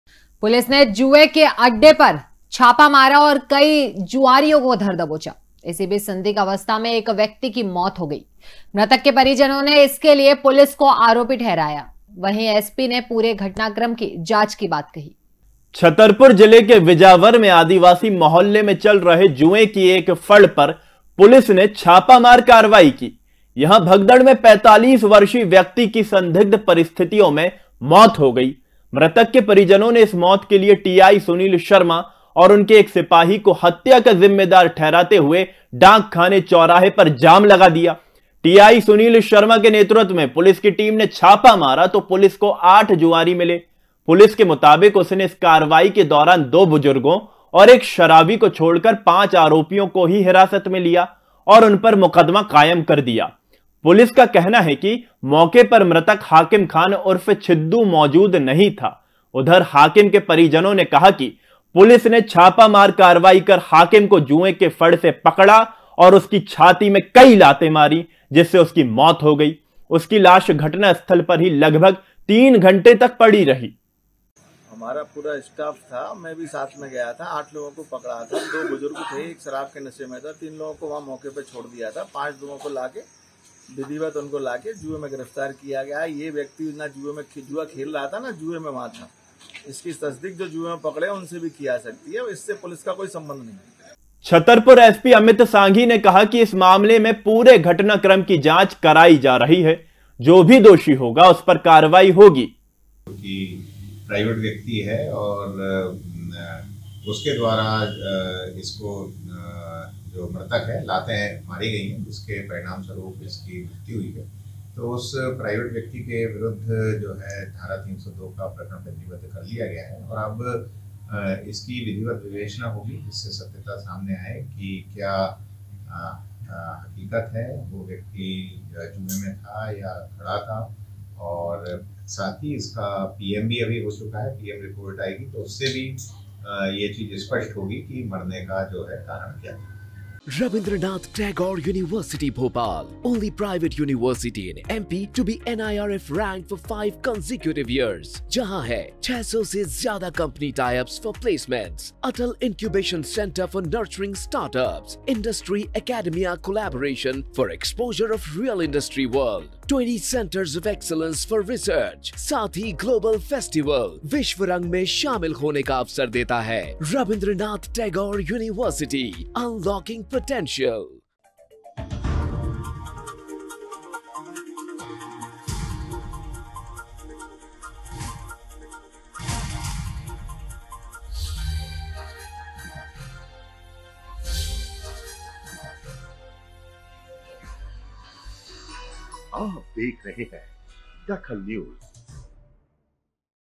DAKHAL NEWS पुलिस ने मारा जुए के अड्डे पर छापा
पुलिस ने मारा जुए के अड्डे पर छापा
एक शख्स की मौत से हुआ बवाल